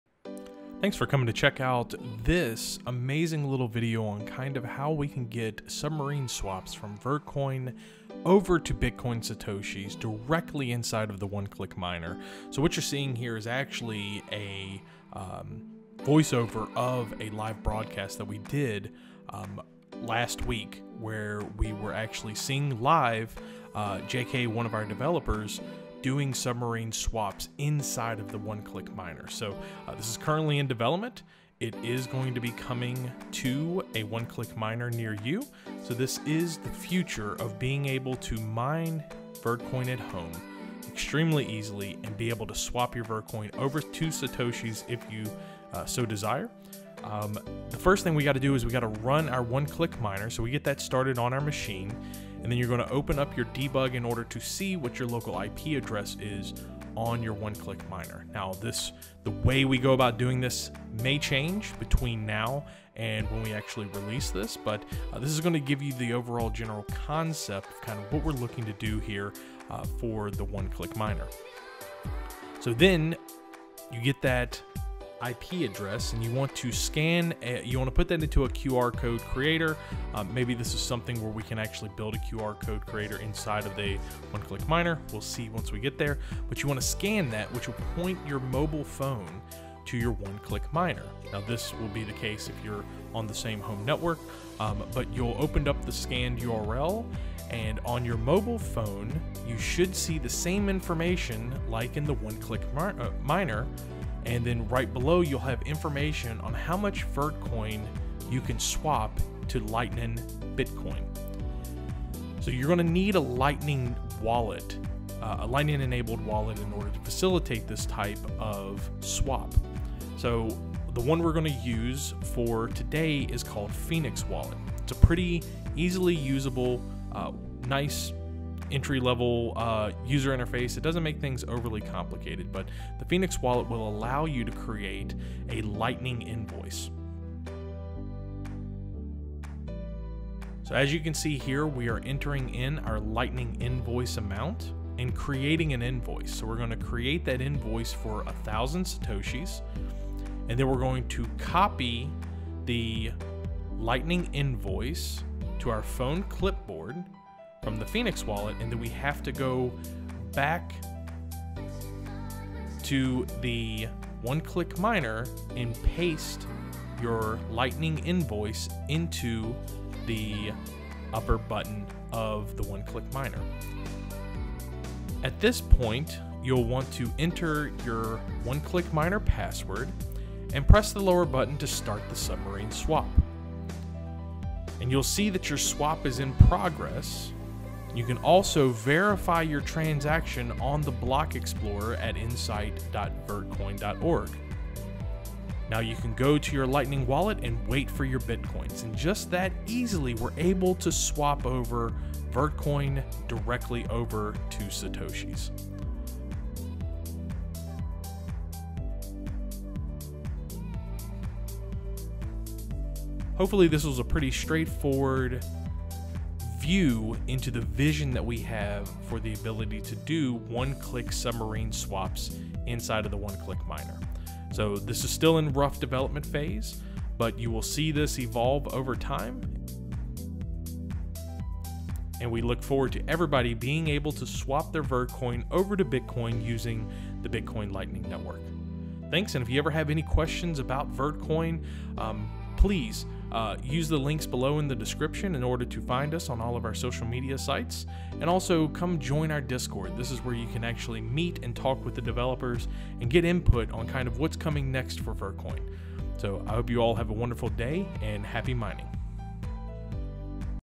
Vertcoin's Submarine Swap Demo in One Click Miner
Vertcoin is the first cryptocurrency in the world to perform a One-Click Submarine Swap. Full symbiosis with Bitcoin. If there is a global adoption of the Lightning Network - it will be thanks to Vertcoin.